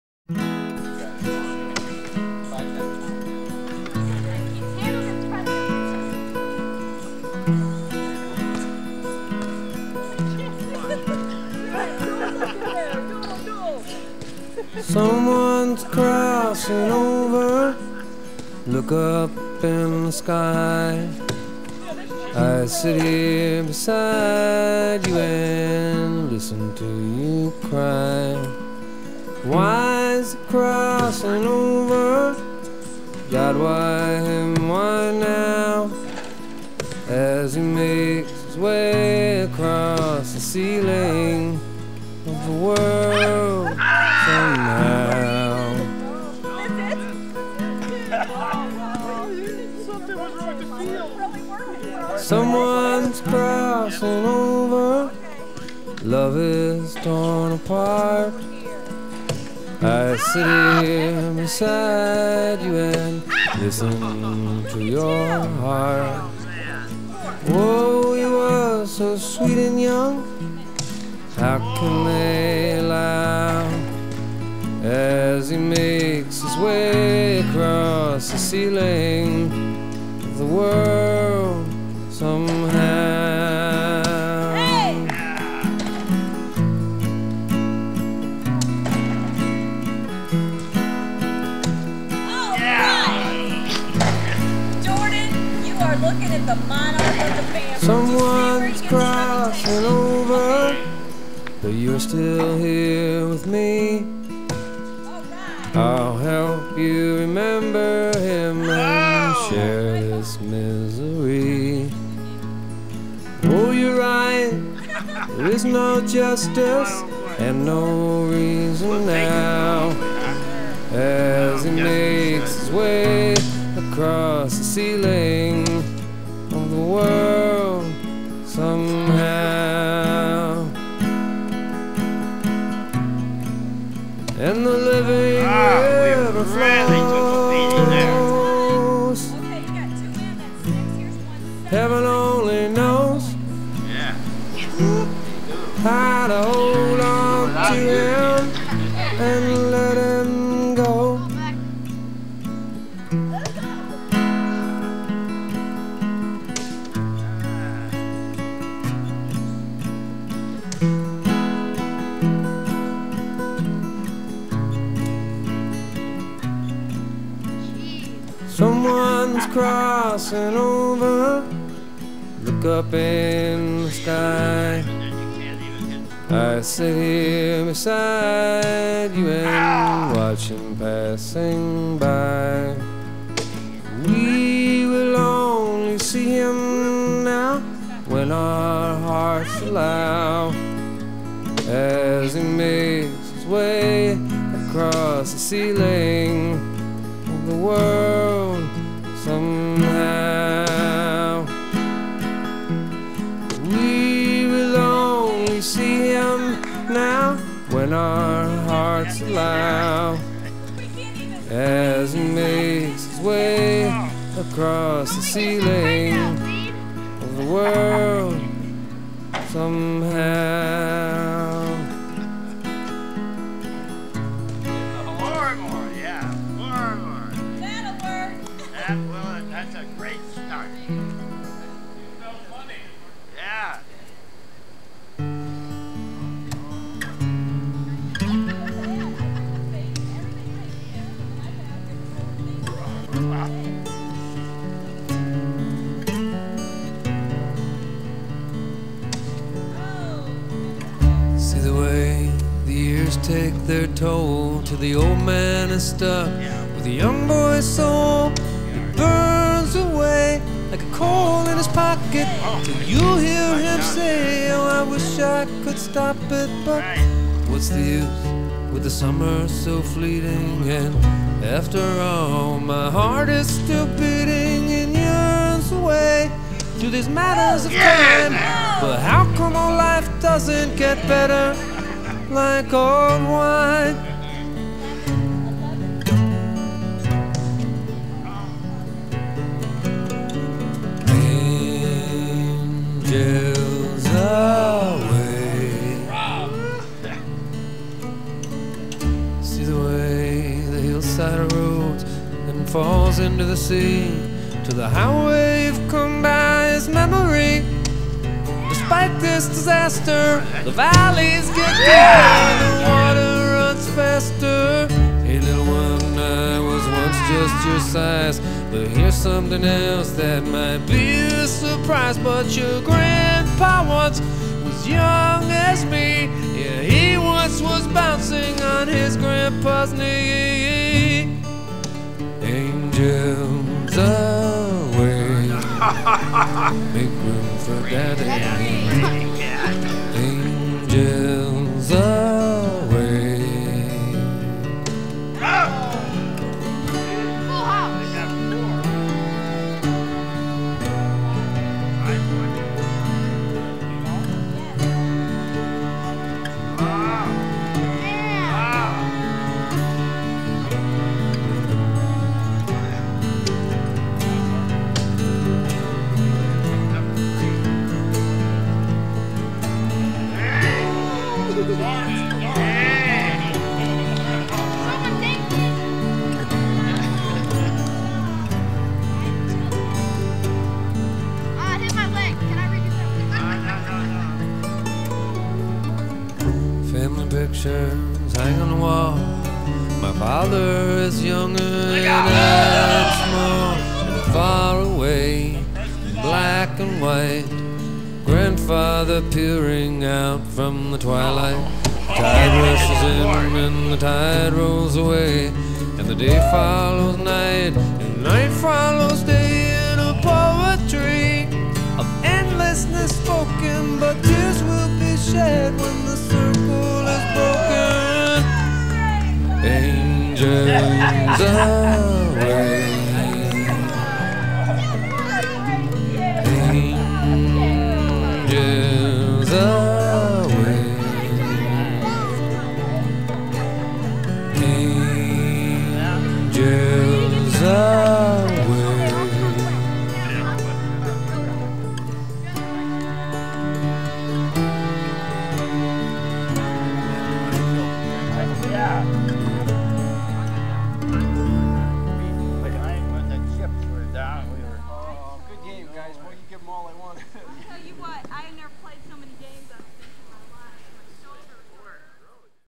Beanbag Dad
Ed's father, Joe Saunders, in his element: reigning beanbag champion at a family reunion.